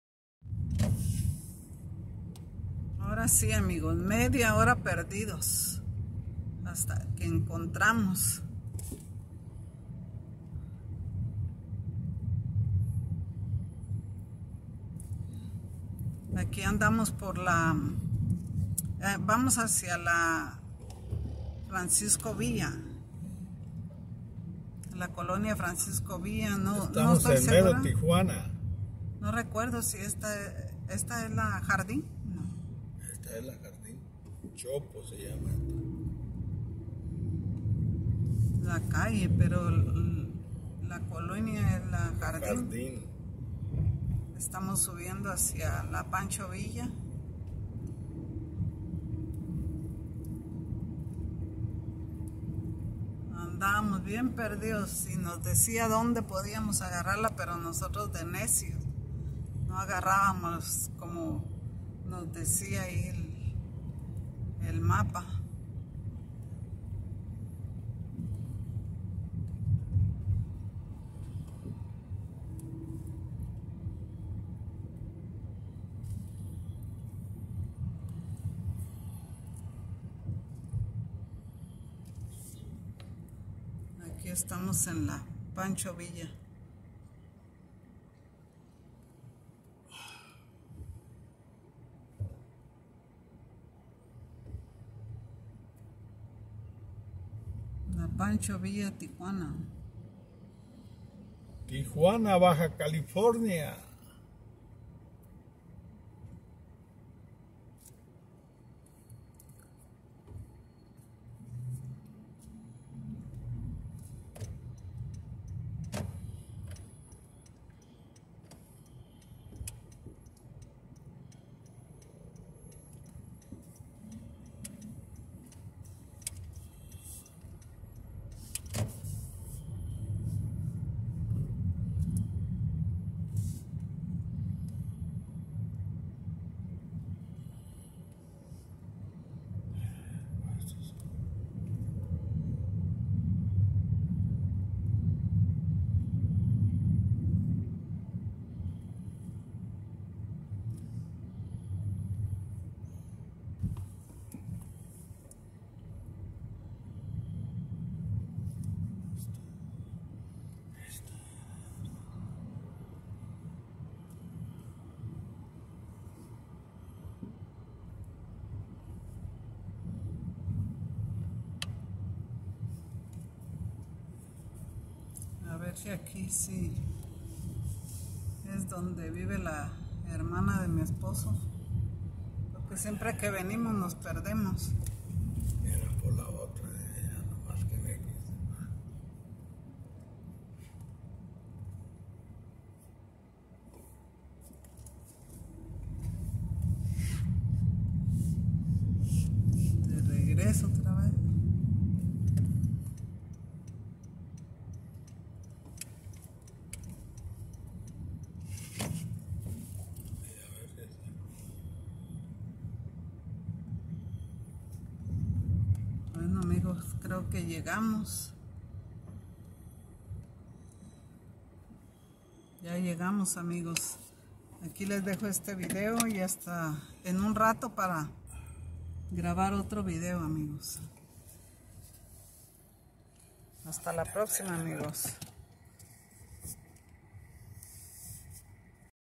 por las calles de Tijuana la Pancho Villa 2021
que diferente se ve todo cuando uno sale de California ahí es colonias de Tijuana